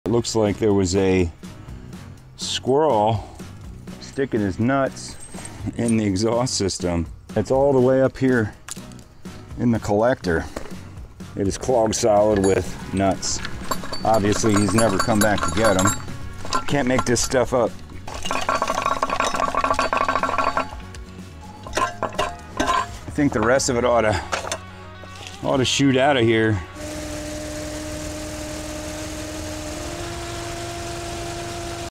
Jeep exhaust is filled with a squirrel nest 😂
Check out the full video of us trying to start this Jeep that has been sitting for 20 years